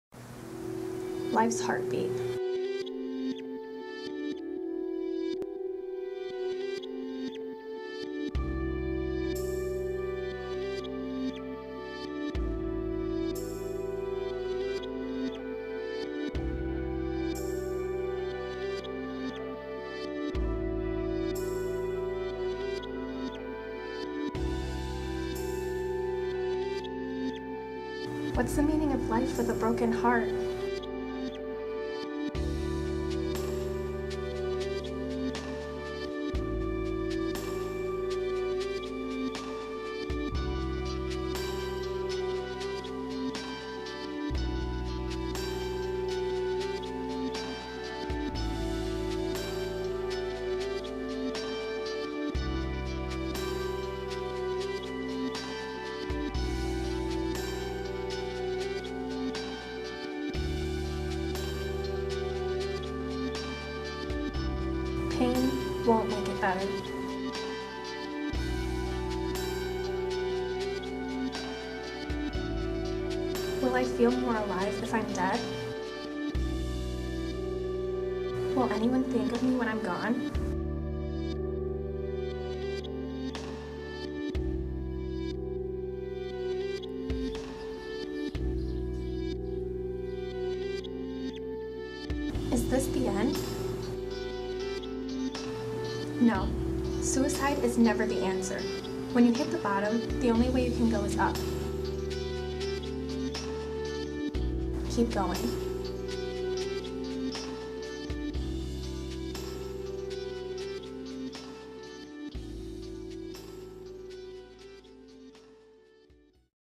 Suicide Prevention- PSA
Suicide is very common in teens. High school can add lots of stress that nobody needs. Treat others the way you want to be treated. Suicide is 100% avoidable.